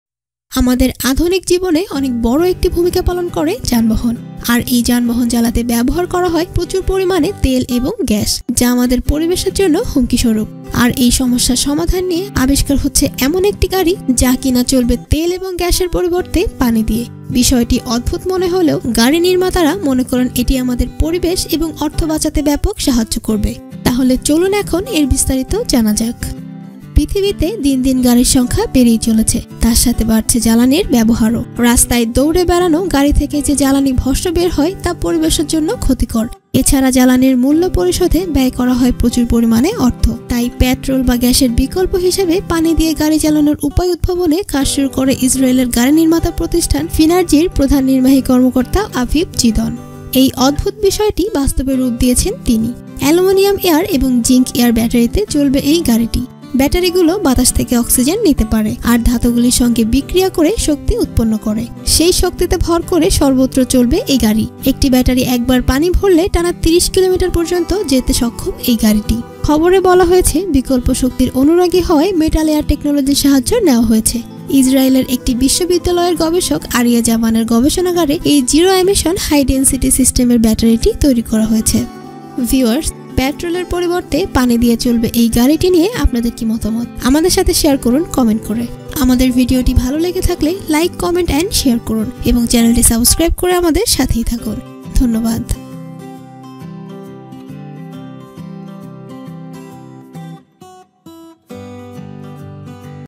তেল-গ্যাস নয়, গাড়ি চলছে পানি দিয়ে! আগামী দিনে গাড়ি চলবে পানিতে।
বাড়ছে জ্বালানির ব্যবহার। সড়কে দৌড়ে বেড়ানো গাড়ি থেকে যে জ্বালানি ভস্ম বের হয় তা পরিবেশের জন্য ক্ষতিকর। এছাড়া জ্বালানির মূল্য পরিশোধে ব্যয় হয় প্রচুর অর্থ।
তাই পেট্রোল বা গ্যাসের বিকল্প হিসেবে পানি দিয়ে গাড়ি চালানোর উপায় উদ্ভাবনে কাজ শুরু করে ইজরাইলের গাড়ি নির্মাতা প্রতিষ্ঠান ফিনার্জির প্রধান নির্বাহী কর্মকর্তা আভিভ জিদন।